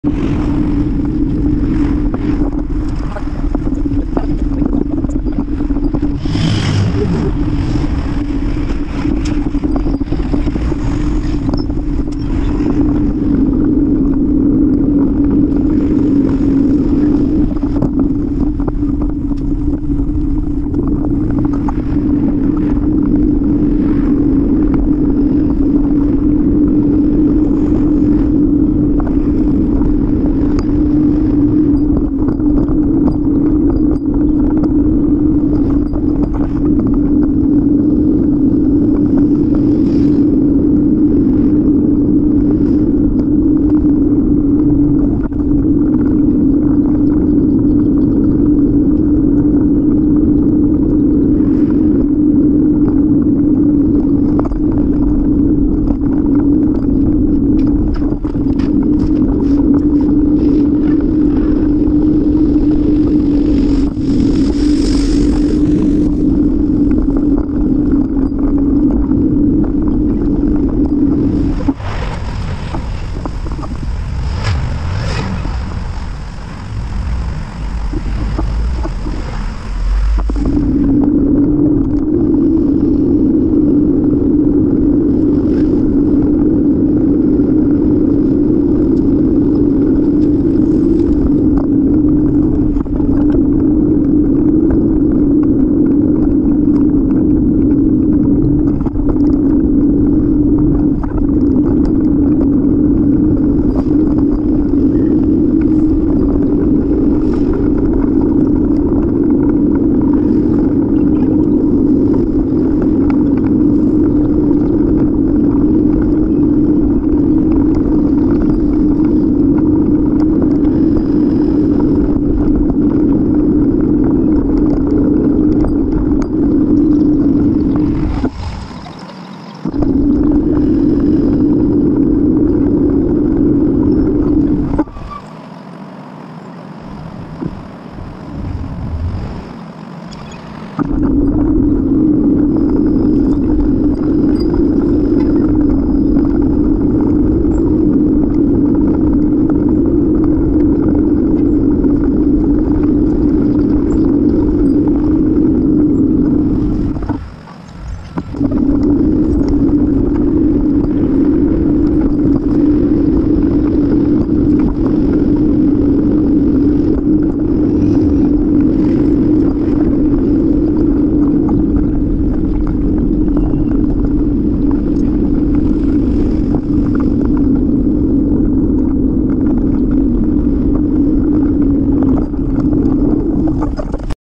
Bike East along Martin Goodman Trail [Timelapse]
Easier ride today. The tires felt flatter already. Might add fix-flat to them at some point.